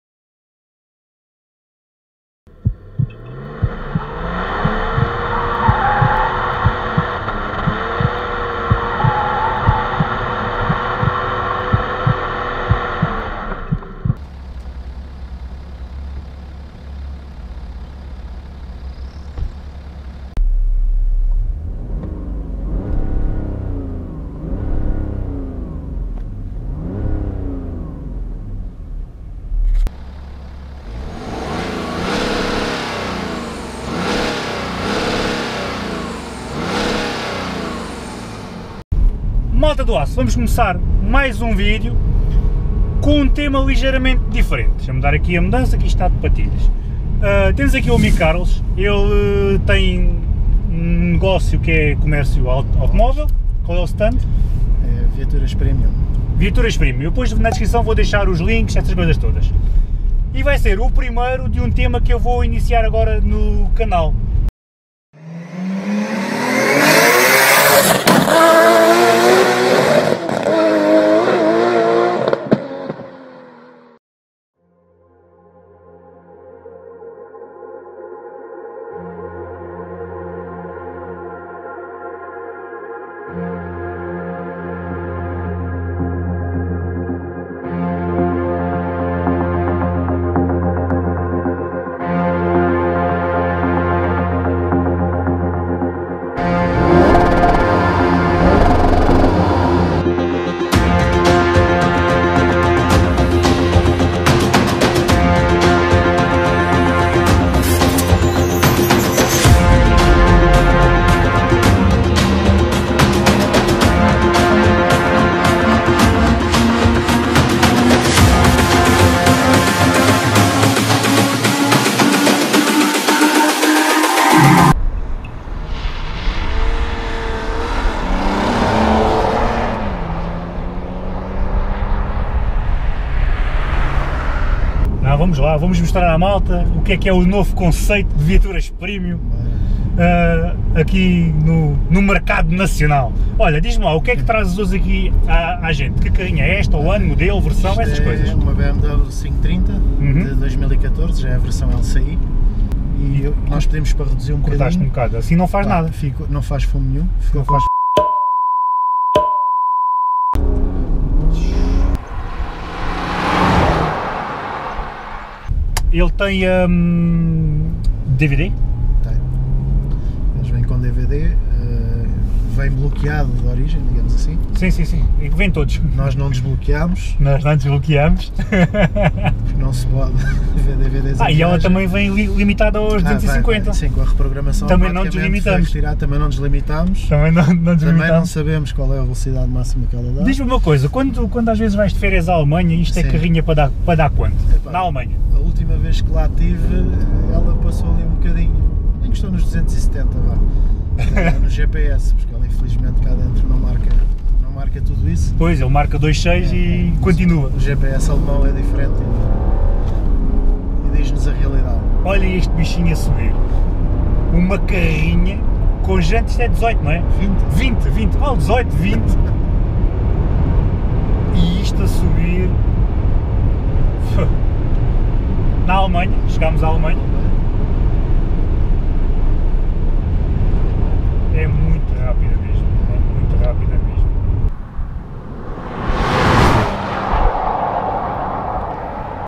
Teaser da BMW 530 FAMILIAR com 700NM´s - Domingo Youtube Yuri Francês
Domingo temos um EXTRA bem LOUCO..

JA imaginaram 700 NM`s de controlo desligado e volante trancado? HEEHEHEHEHEH

Malta este è o tal video que vos falei da BMW da CP AutoMotive.

NA altura que filmamos este video nem o Stand se chamava CP AutoMotive e nem  Carlos pensava que ia ser meu patrocinador do CUP.

Felizmente as coisas correram bem mesmo sem lançar este video.
A parceria foi fechada no inicio do mes muito antes de sair o video.

Pedia que passassem pelas redes do Carlos e deixassem o vosso LIke.. Ajuda IMENSO .